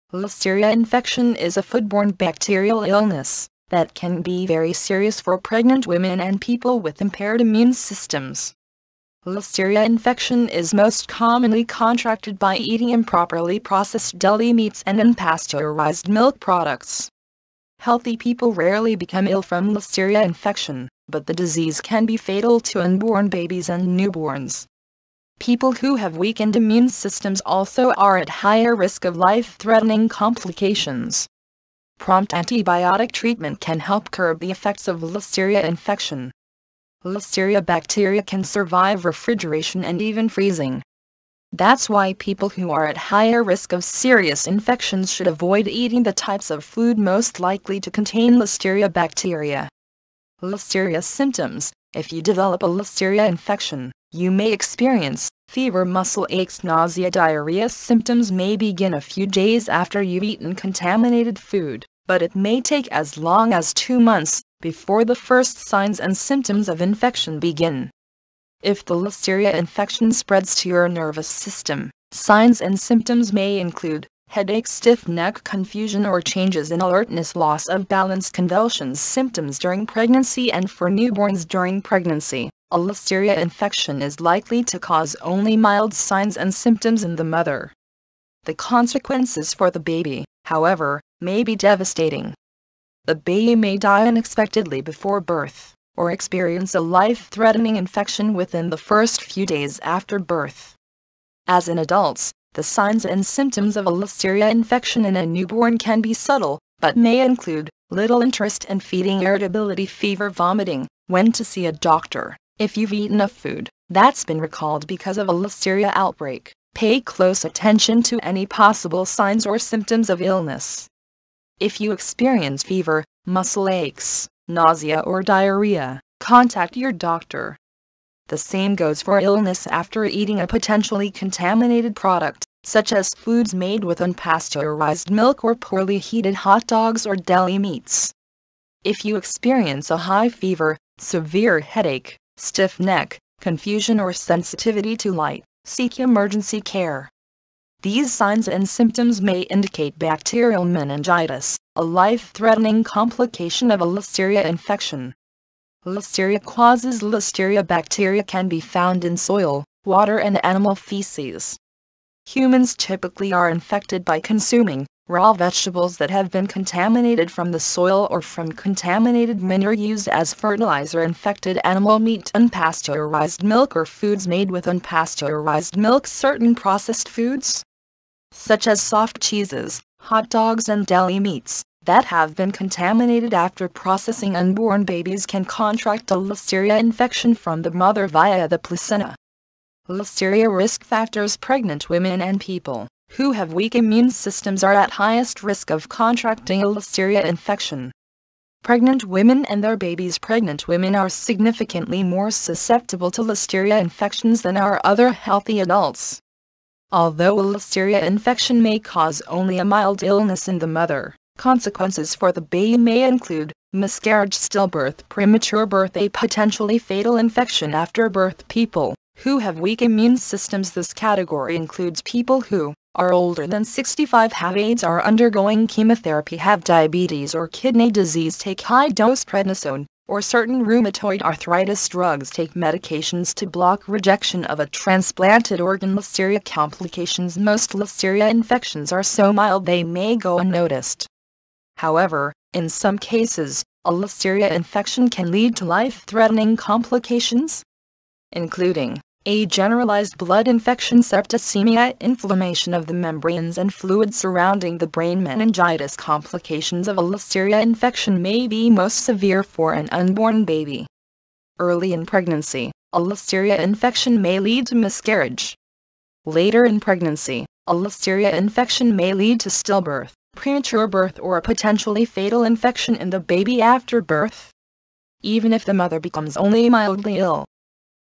What is Listeria Infection and Its Symptoms
Listeria Causes, Risk factors and Complications